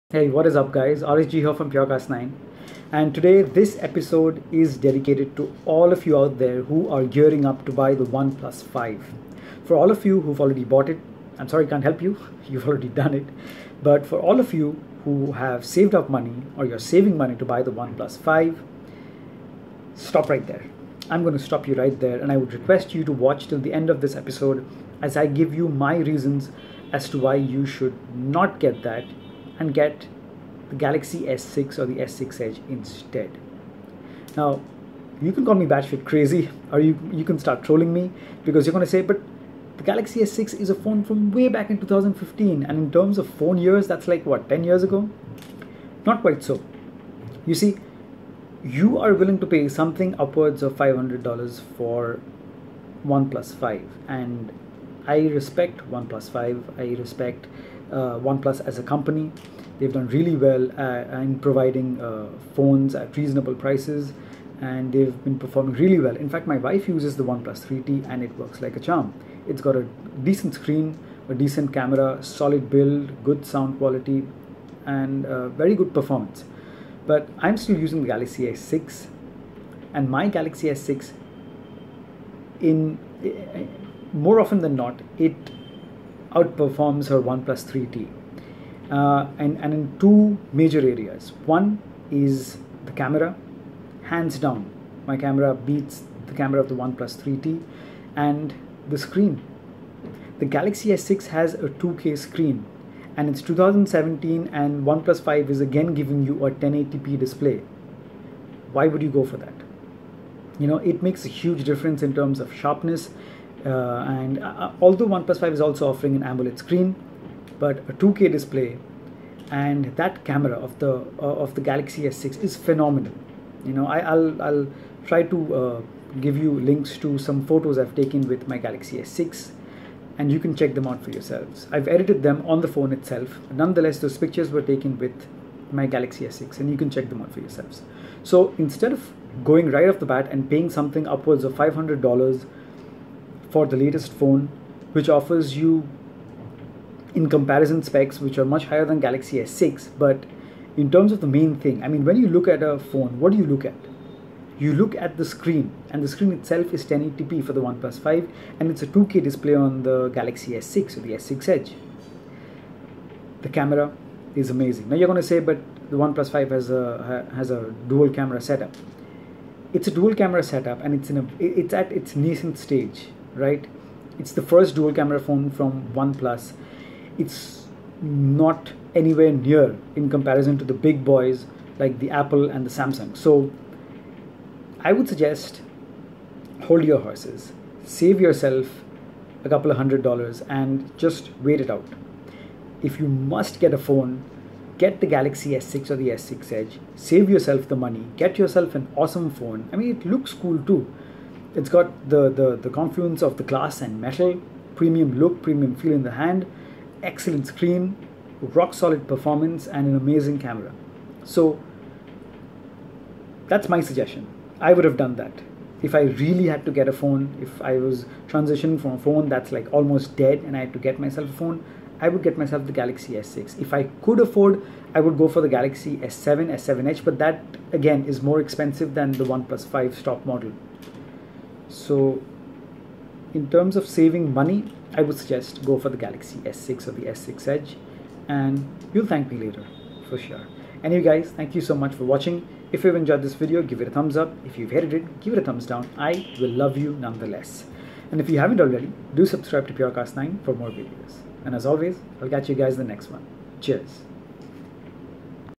DON'T BUY THE OnePlus 5 ! ! ! Get THIS instead.
Before you spend your precious money on the OnePlus 5,just give me 5 minutes of your time and I will convince you to get something way better for much less,I guarantee it!

Cheers!